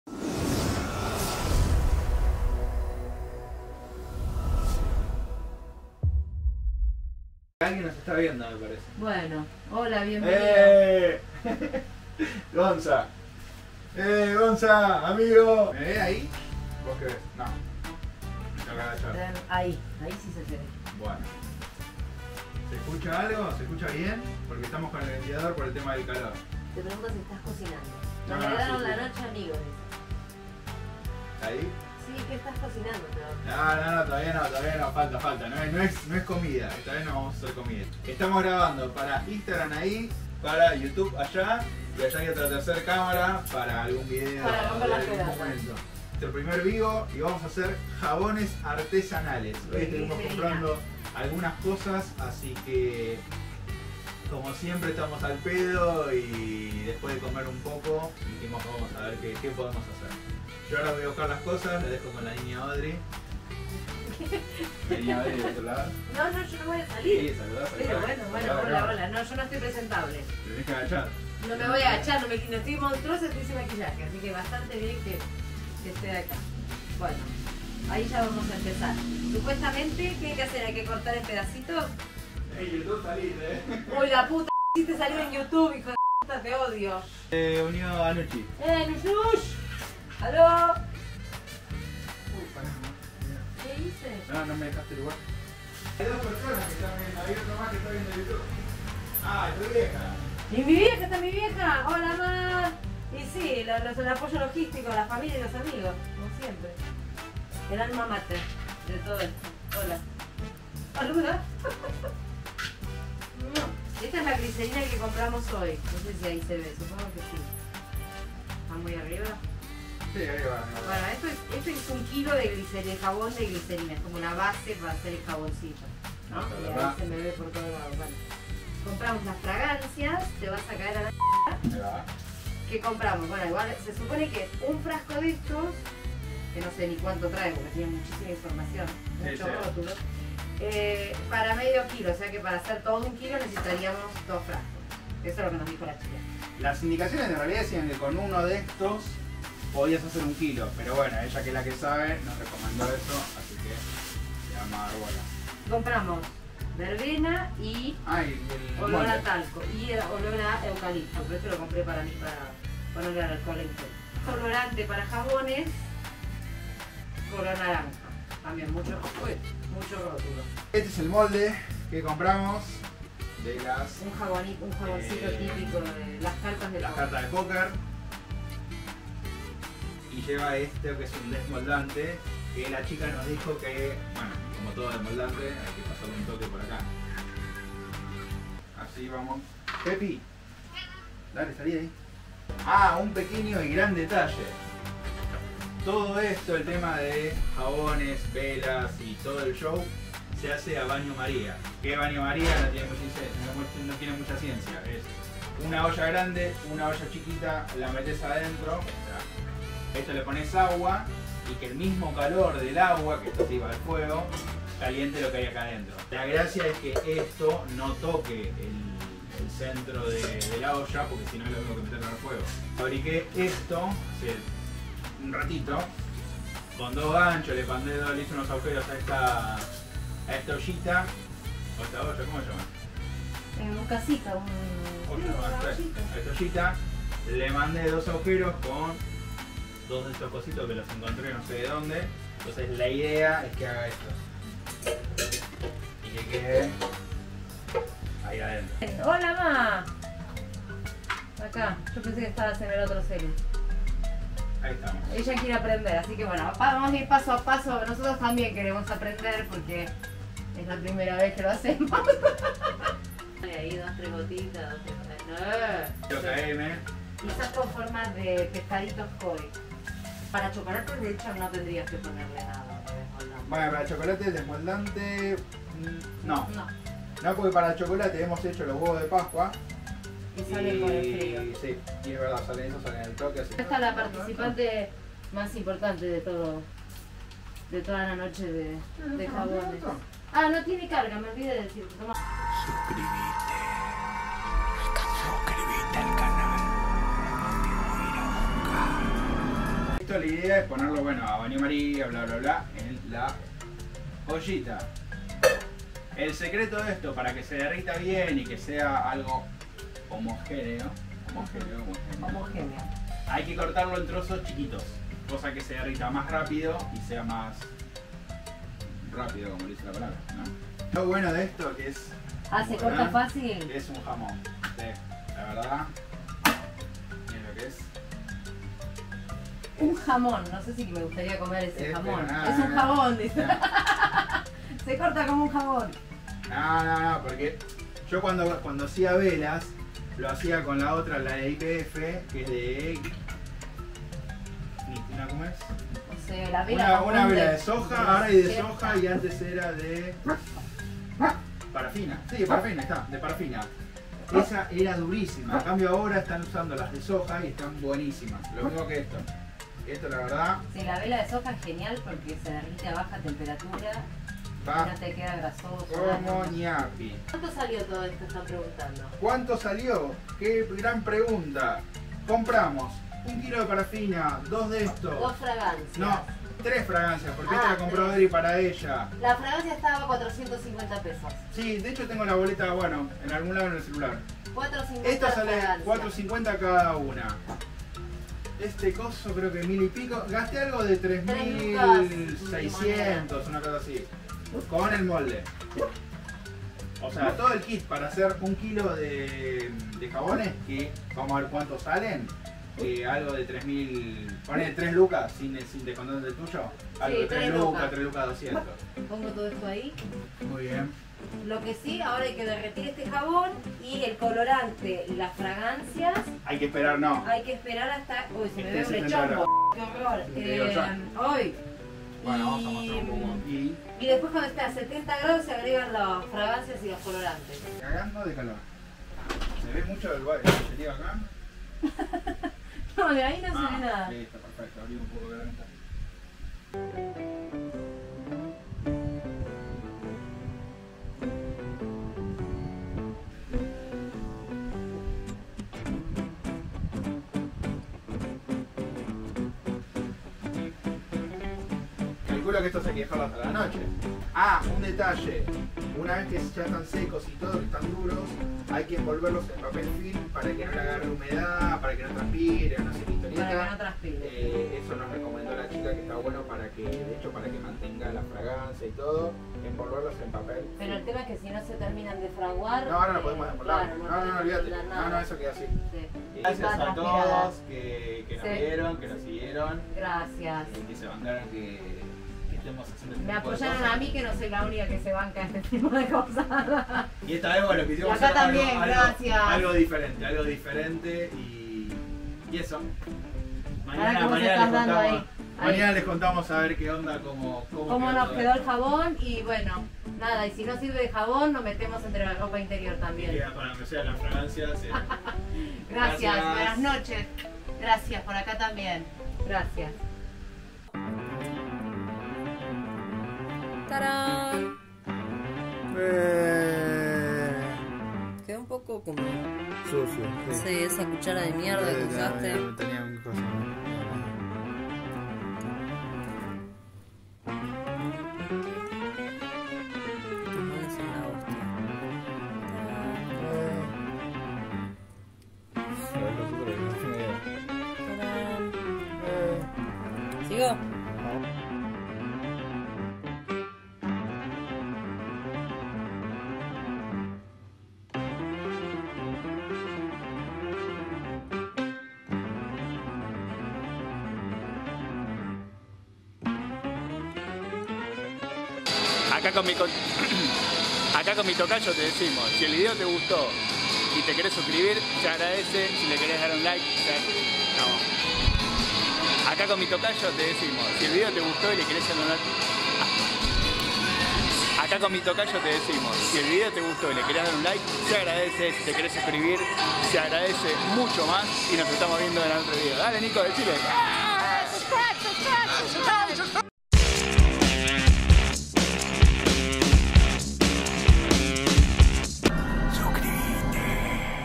🧼JABONES 🧼Artesanales de GLICERINA Paso a Paso GRABADO con CÁMARA TESTIGO!!
Espero que el video les guste,

Este video salio de casualidad un sábado por la noche. Fue los primeros jabones que hicimos y la pasamos súper bien transmitiendo en VIVO!!!